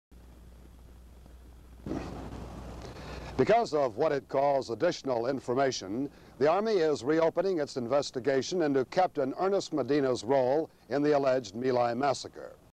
wsbn61342.mov
Excerpt from wsbn61342. See full clip @

.Army reopening case into Medina's role in My Lai massacre.